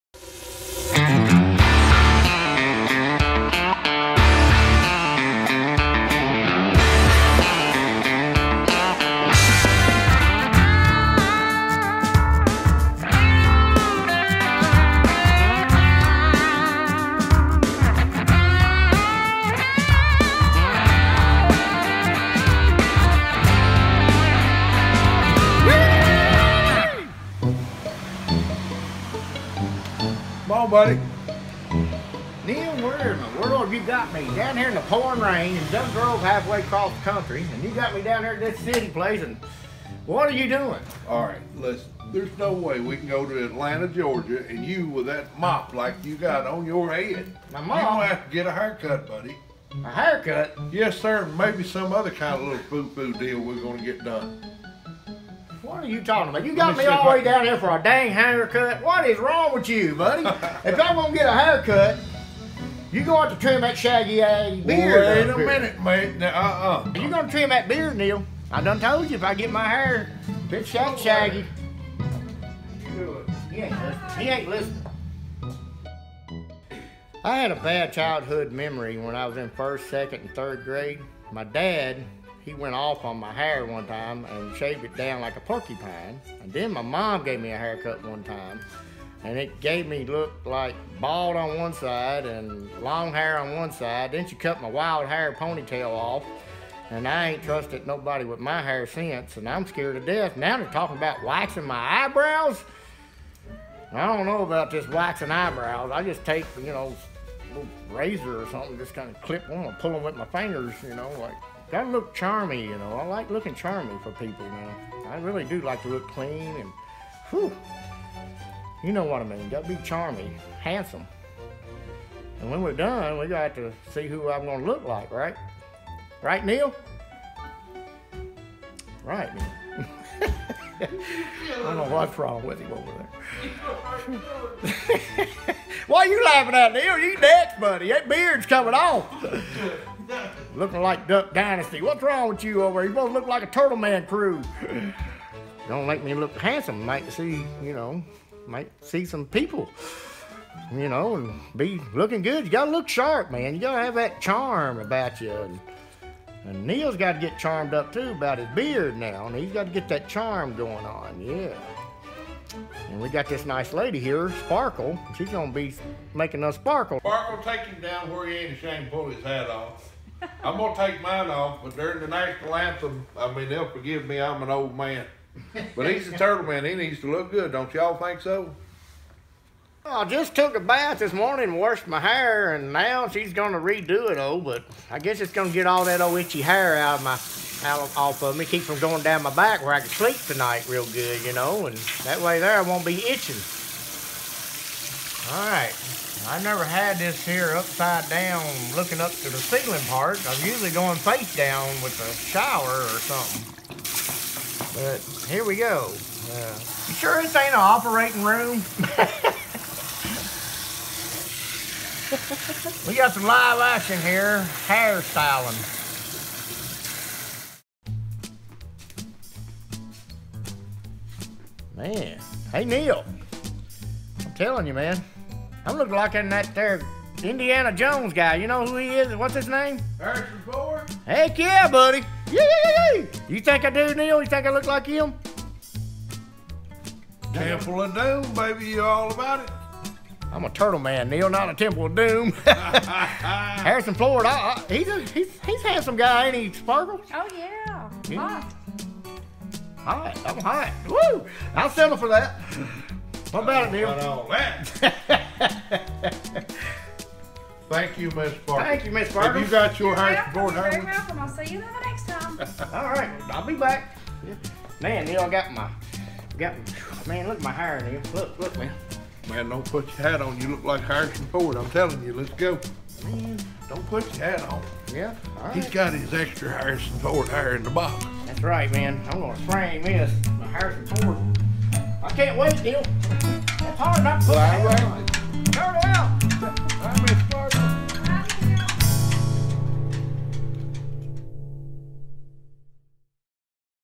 Turtleman gets a haircut
Before Turtleman and Banjo Neal went on a recent road trip, Neal tricked Turtleman into getting a haircut at what they called a "Foo-Foo" Hair Salon!